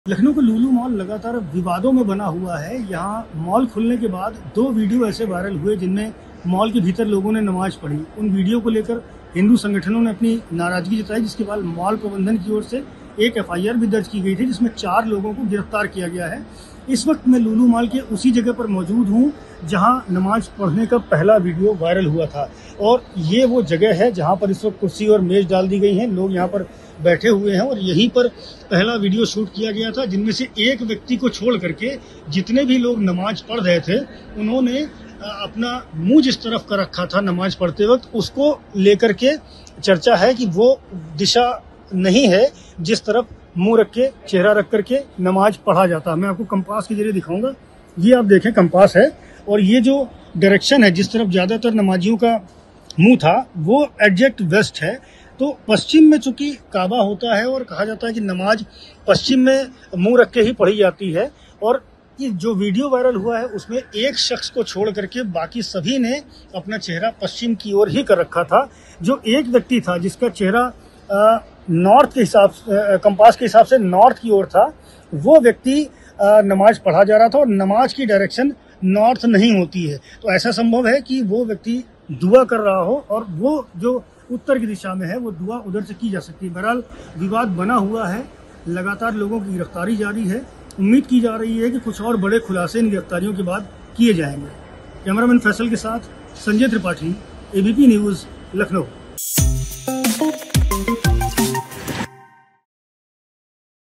क्या वाक़ई में पढ़ी गई थी Lucknow के Lulu Mall नमाज़ ? देखिए ABP News की खास पड़ताल
लखनऊ के लुलु मॉल में नमाज़ के वीडीयो सामने आने के बाद abp news ने मॉल के भीतर जाकर इस बात को परखने की कोशिश की कि जो युवक नमाज़ पढ़ने आए थे, वो वाक़ई नमाज़ ही पढ़ रहे थे या ये कोई शरारत या साज़िश थी। तीसरे फ़्लोर पर उसी जगह पर हमने कंपास से यह जाँचा कि क्या काबा की दिशा यानी पश्चिम दिशा की ओर नमाज पढ़ने वालों का मुँह था कि नहीं?